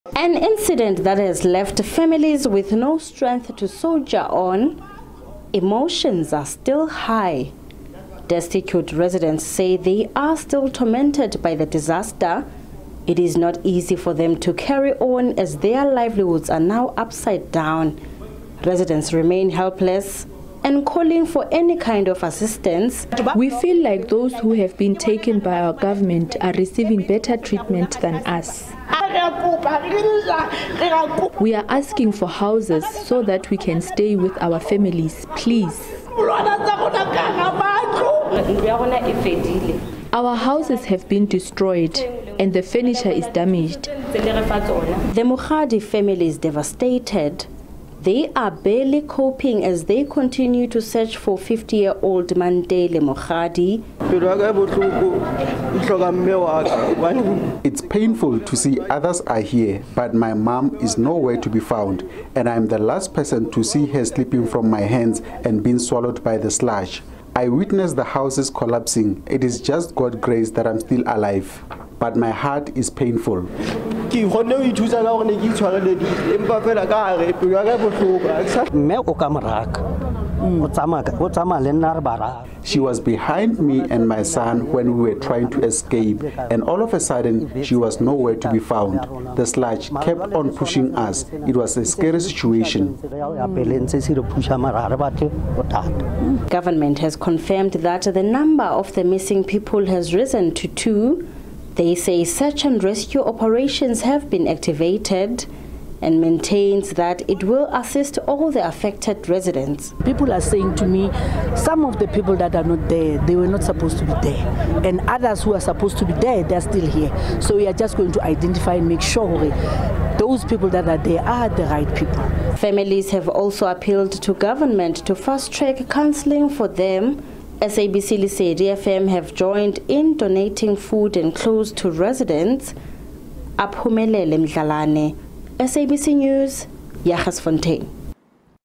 Calls for Free State Government to help Jagersfontein families
Some displaced families in Jagersfontein are calling on the Free State government to assist them. They say they've been helpless since disaster struck last Sunday, when a mine dam burst, destroying their homes.
Meanwhile, the Mokhali family is still searching for missing 50-year-old Mantele Mokhali,
She was last seen moments before the disaster.